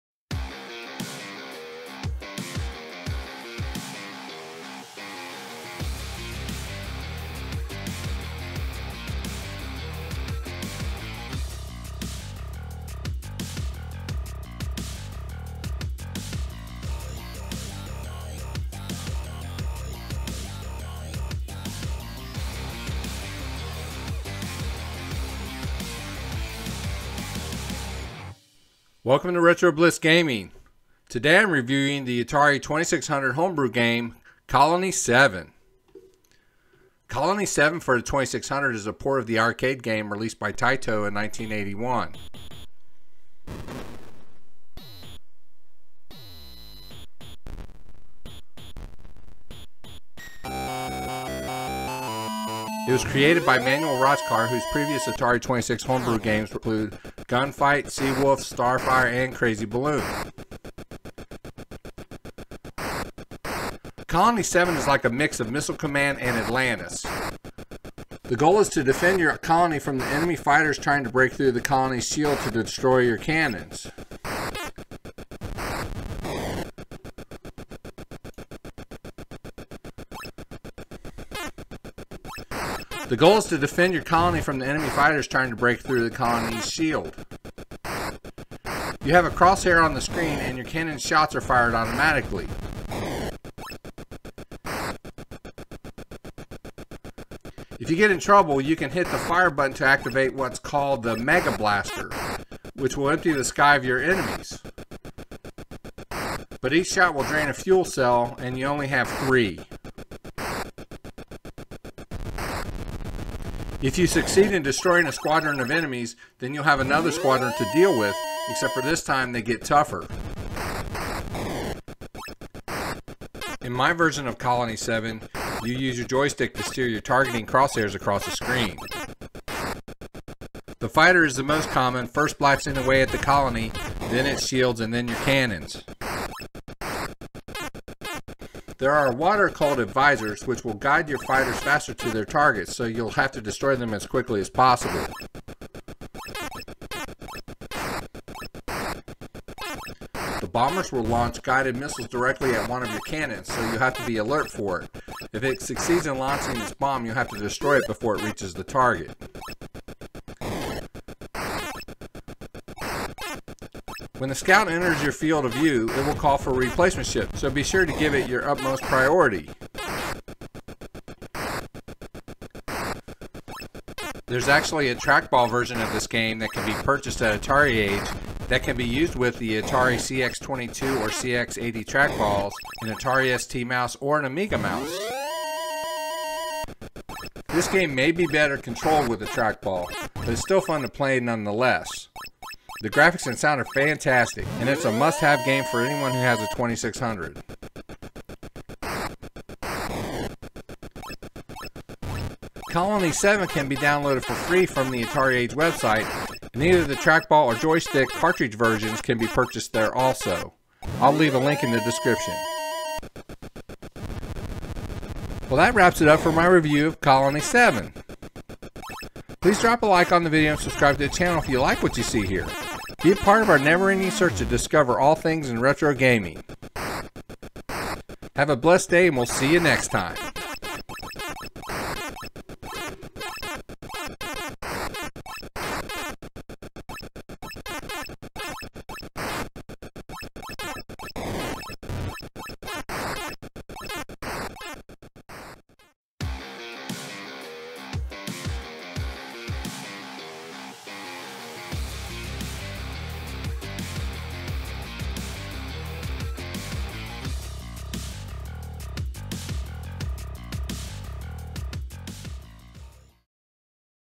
Colony 7 - Atari 2600 Homebrew Game Review
This is a review of the Atari 2600 homebrew game Colony 7, which is a port of the arcade game by the same name.